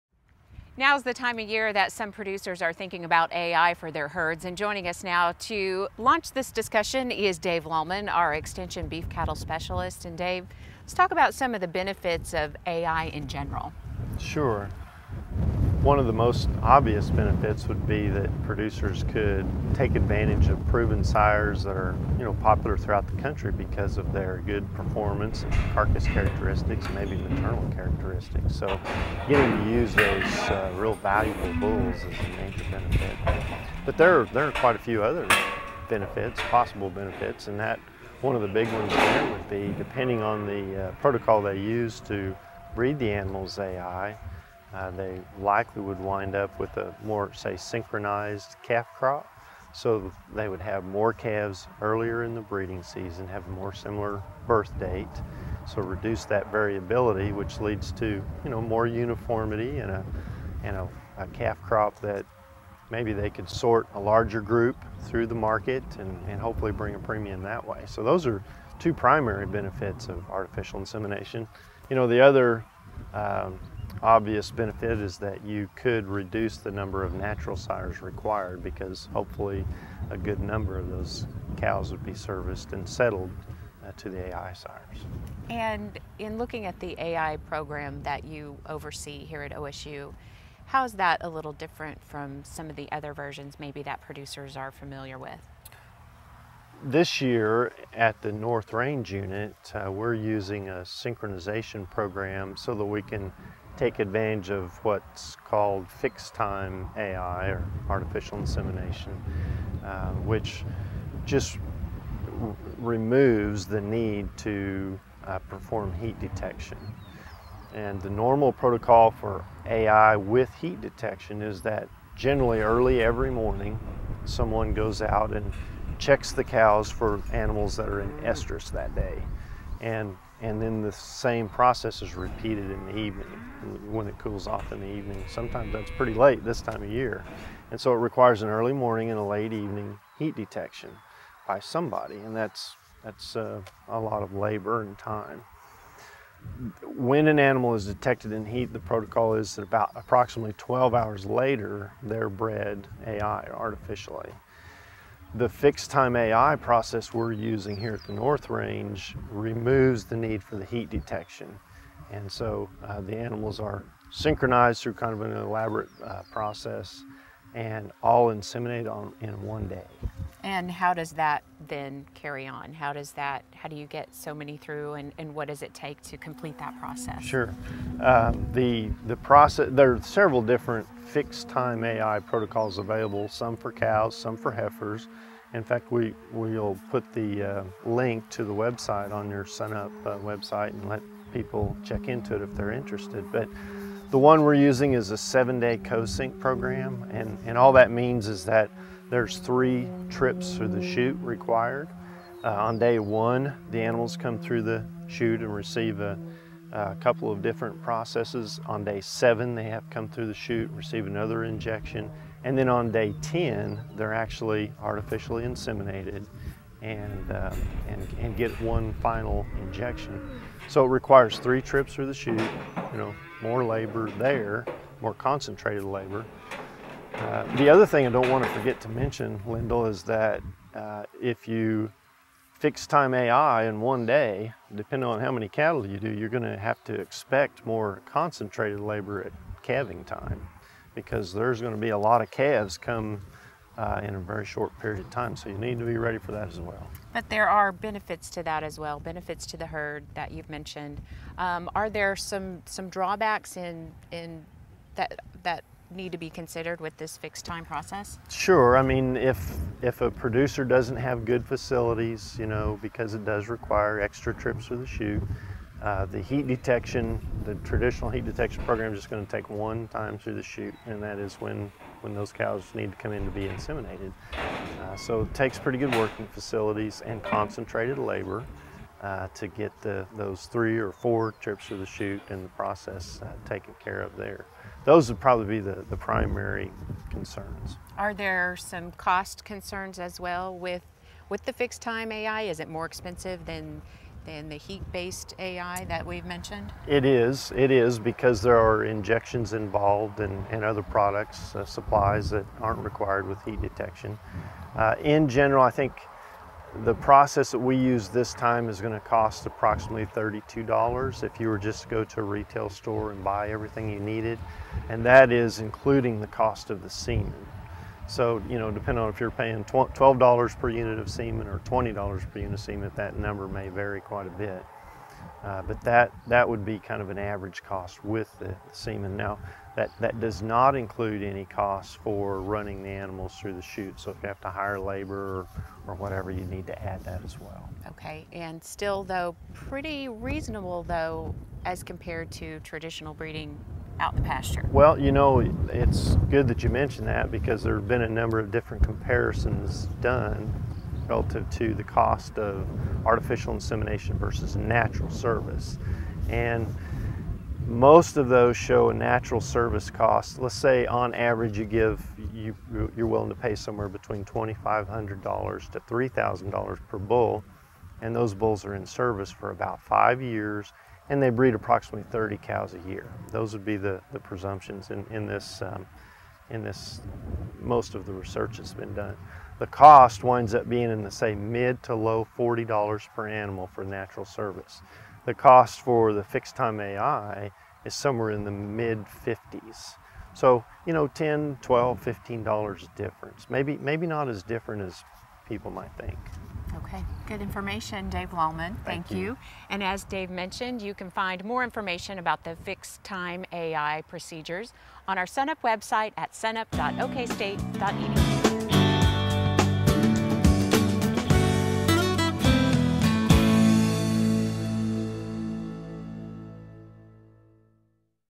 Artificial Insemination for Cattle
SUNUP's Lyndall Stout talks with OSU Extension beef cattle specialist Dave Lalman about the benefits of artificial insemination and a few of the protocols used for AI.